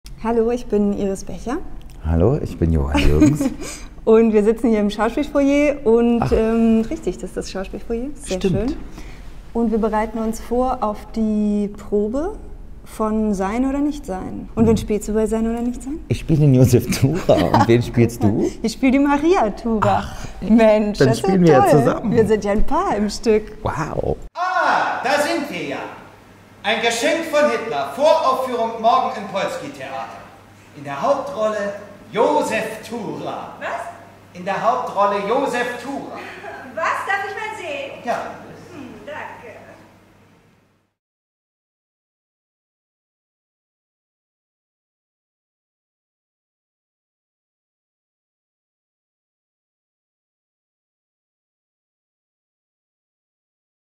SEIN ODER NICHTSEIN // Premiere am 9. Oktober 2021 im Schauspielhaus
Schauspieler:innen Iris Becher und Johann Jürgens über ihre Rollen in "Sein oder Nichtsein".

Sein oder Nichtsein
Komödie von Nick Whitby nach dem Film von Ernst Lubitsch, Drehbuch von Edwin Justus Mayer und Melchior Lengyel

Regie: Christian Weise
Bühne und Kostüme: Paula Wellmann
Licht: Oskar Bosman
Dramaturgie: Dirk Baumann

Premiere: 9. Oktober 2021, 19:30 Uhr, Schauspielhaus